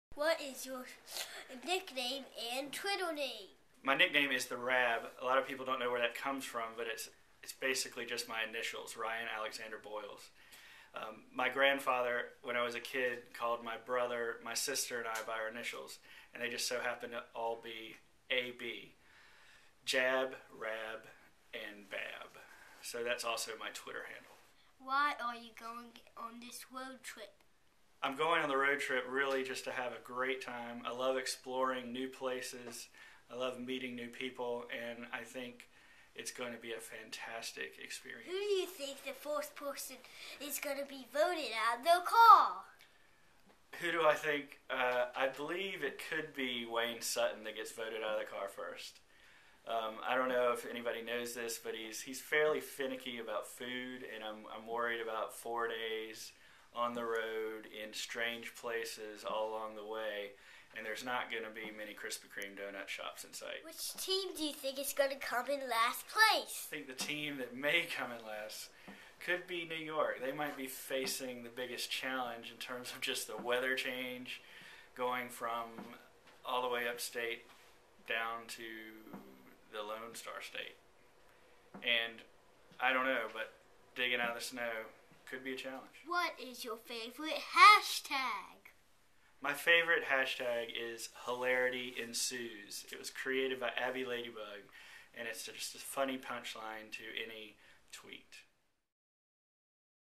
My son interviews me for Chevy SXSW Road Trip
Follow our road trip adventure from Raleigh / Charlotte, North Carolina to Austin, Texas on the Team NC travel blog